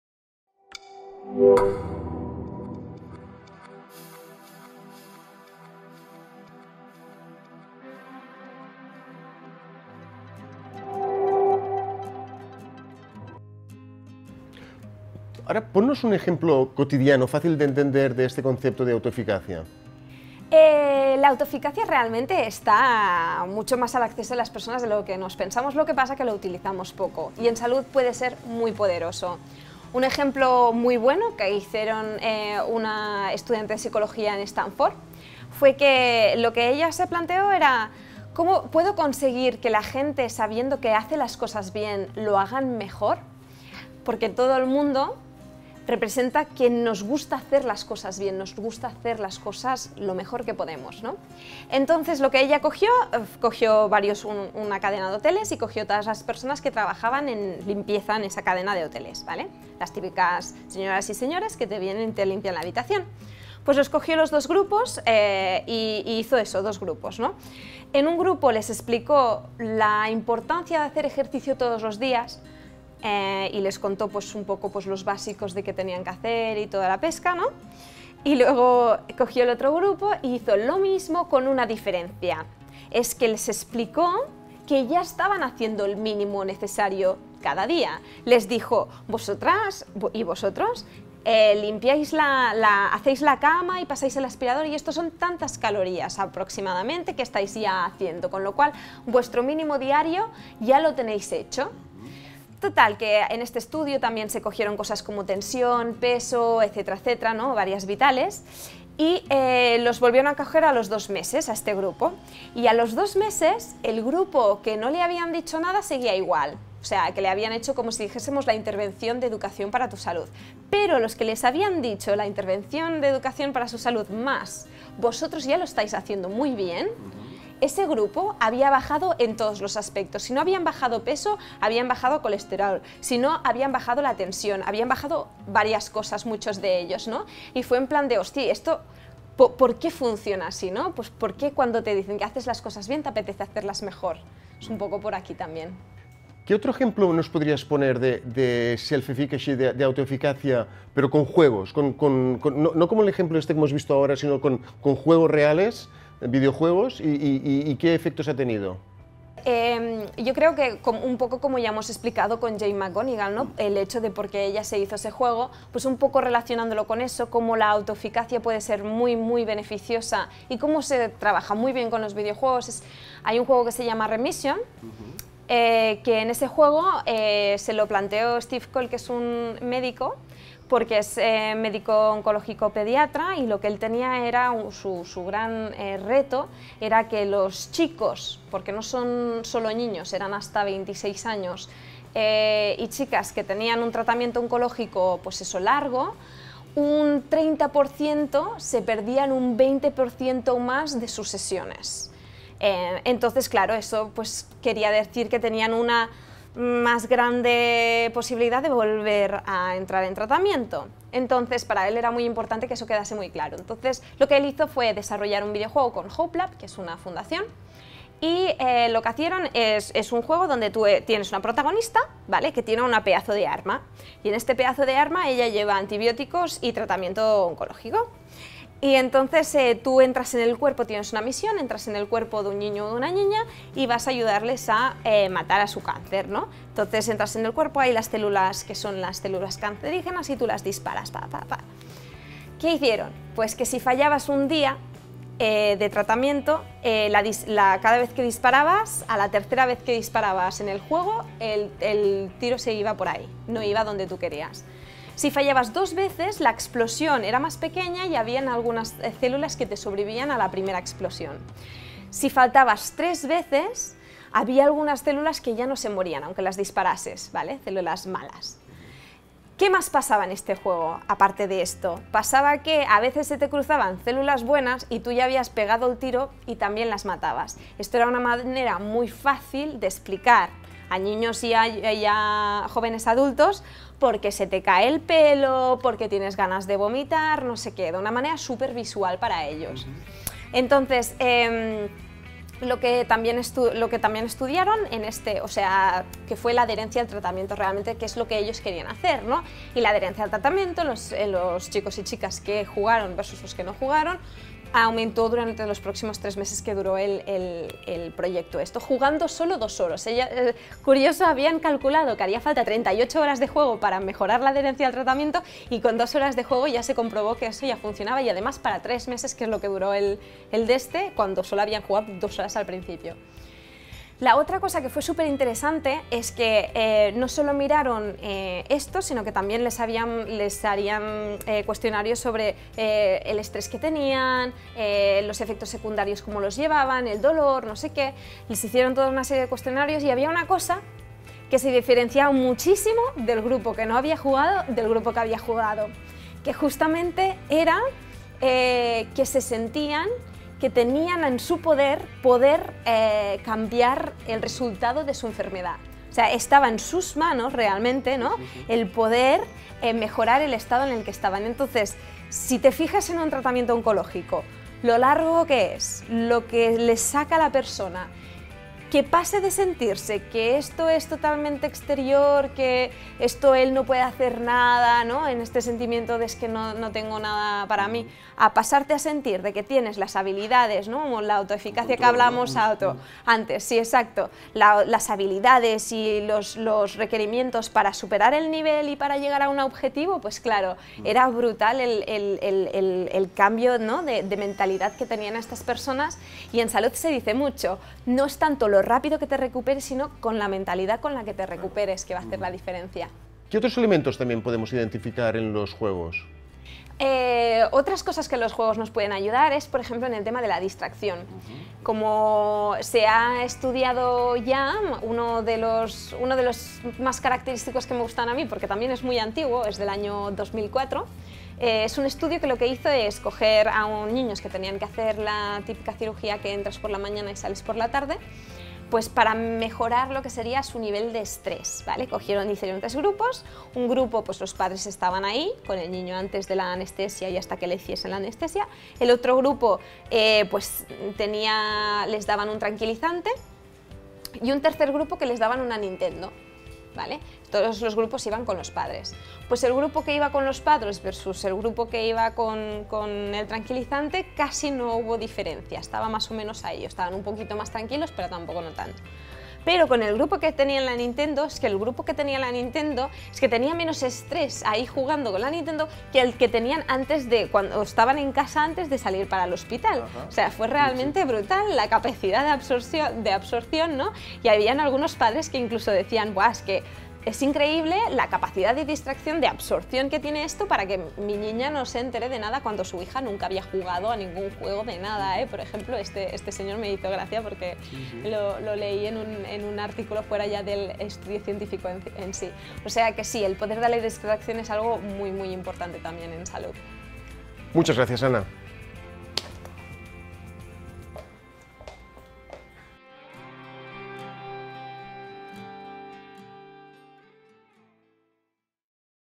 Salud y autoeficacia
Curso de Introducción a la gamificación a través de casos prácticos
El concepto de autoeficacia explica parcialmente la potencia de los juegos a la hora de motivar.

Más información sobre el curso de Introducción a la gamificación a través de casos prácticos de la UOC: